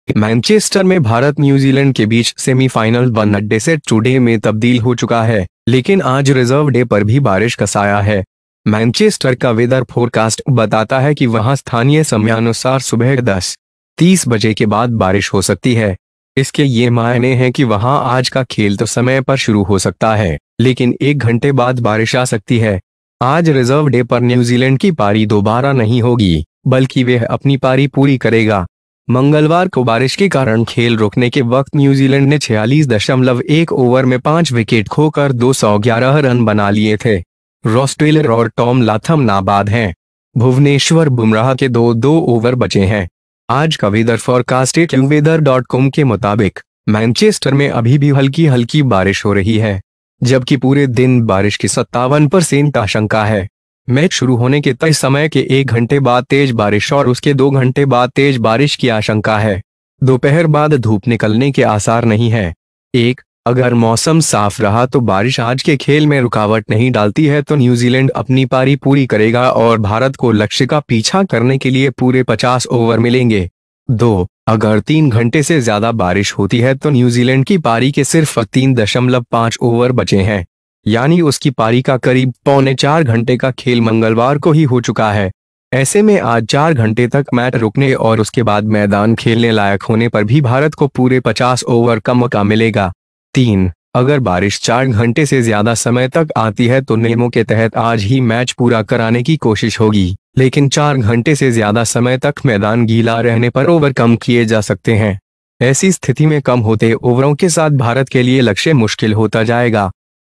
मैनचेस्टर में हल्की बारिश, मैच समय पर हुआ तो न्यूजीलैंड 461 ओवर से खेल शुरू करेगा
In Manchester, India-New Zealand has been transformed from semi-final one-day to two-day, but it is also rainy on the Reserve-Day today. The Weather Forecast of Manchester suggests that there may be rain after 10.30 in the morning at the local time. It is worth mentioning that today's game can start on time, but it can get rain after one hour. Today, New Zealand's innings will not be revisited on the reserve day, but it will complete its innings. On Tuesday, when the game stopped due to rain, New Zealand had scored 211 for 5 wickets in 46 over 1 over. Ross Taylor and Tom Latham are unbeaten. Two-two o'clock in Bhubaneswar-Bumrah are left. According to the Weather For Acquired Dot Com Today, there is still slight rain in Manchester. While there is a 57% apprehension of rain all day. After an hour of scheduled time of start of the match, heavy rain and two hours after the rain is expected. There is no hope of sunburn after noon. 1) If the weather is clear then the rain does not obstruct the game today, New Zealand will complete its innings and India will get 50 overs to chase the target. 2) If it rains more than three hours then New Zealand's innings Only 3 decimal 5 are left over. That is, about four and a half hours of his innings have happened on Tuesday. In such a situation, for four hours to stay till the match and after playing field, India will get a full 50 overs. 3) If the rain rains for more than four hours, then under the rules, , But for more than four hours, the overdose can be reduced if the field is wet. In such a situation, the target for India will be difficult with reduced overs.